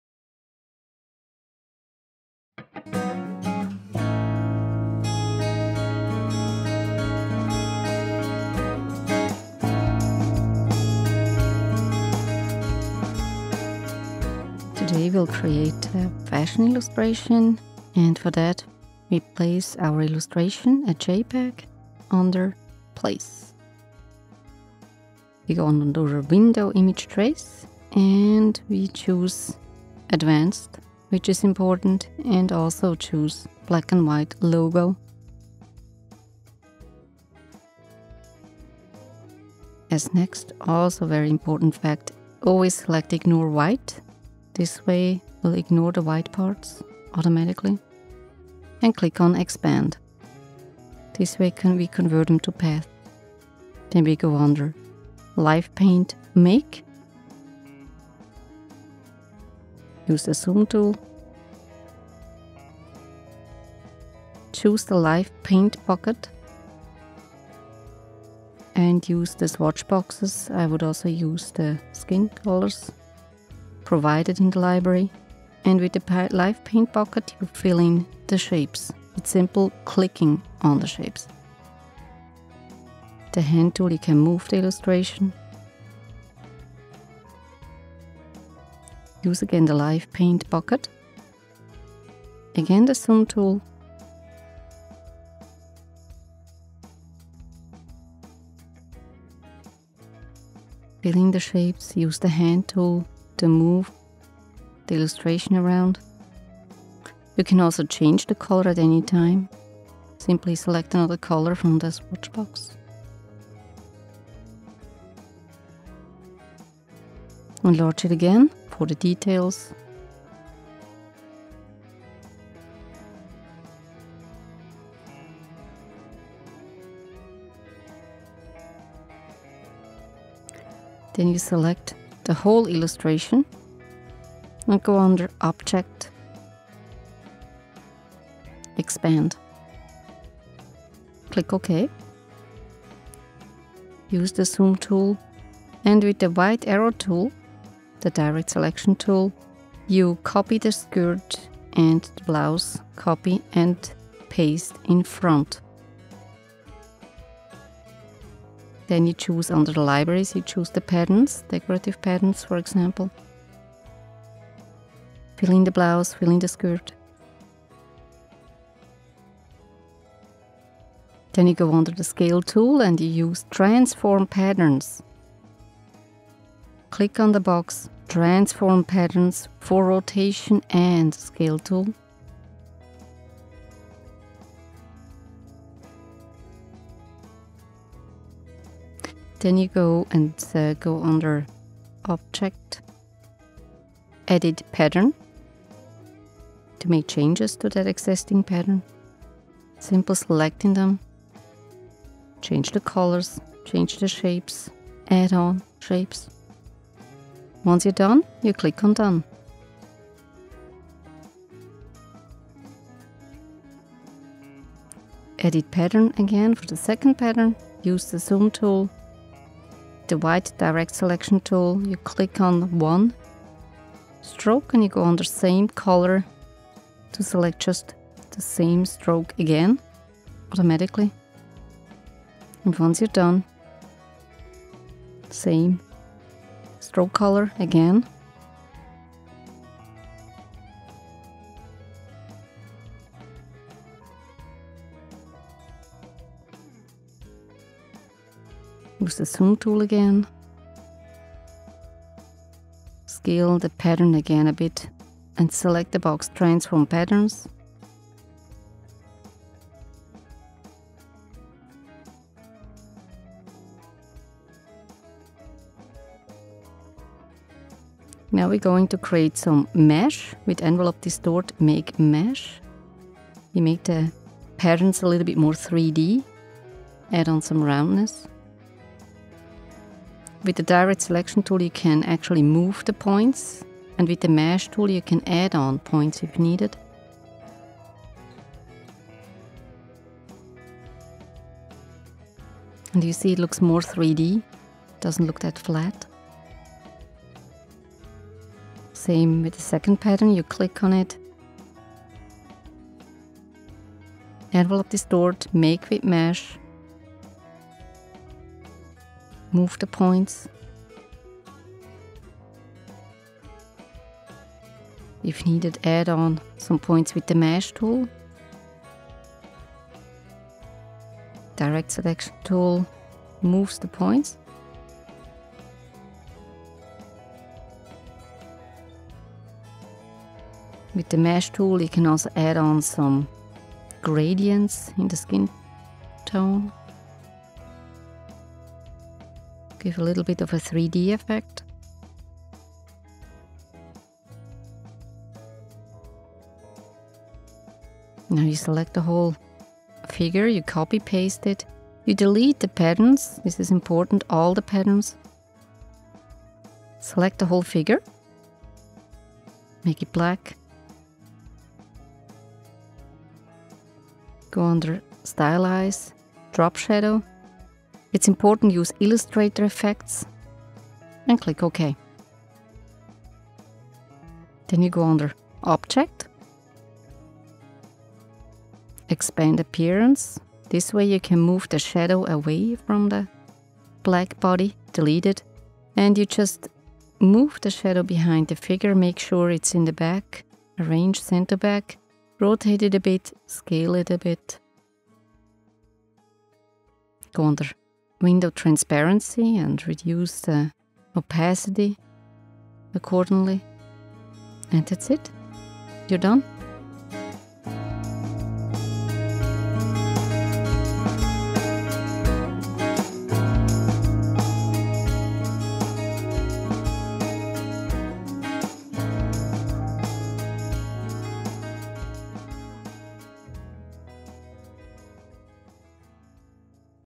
Fashion Illustration - Tutorials at: i-adobe.com
How to use the image trace option, to create and alter patterns, the life-paint option for quick coloring and the mesh-tool for 3D effects - in Adobe Illustrator CS6 - for intermediate users -
- Wie man mit dem Bildnachzeichner arbeitet, die Interaktiv-malen Option und das Verlaufsgitter um 3 D Effekte zu simulieren - für mittleres Niveau -

presented by Miriam Selmi Reed / REEDesign.ch. Bilingual instructions in English and German subtitles presented by Miriam Selmi Reed & Michael Alan Reed /
Zweisprachige Anleitung in Englisch mit deutschen Untertiteln. Copyright by Mike Reed & Miriam S. Reed